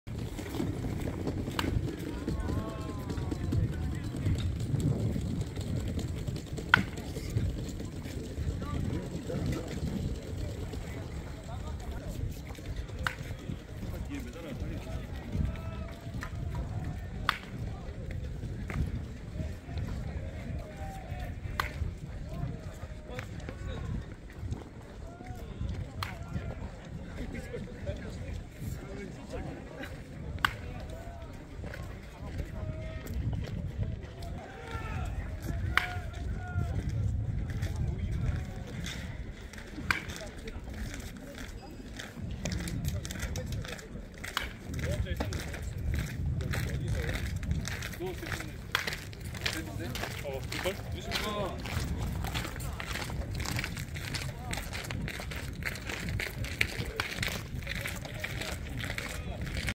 인천 동산고등학교 야구부 타자들 배팅 프랙티스 영상 x 고교야구 동계훈련
고교야구 동계훈련 현장체크 x 더 베이스볼 팩토리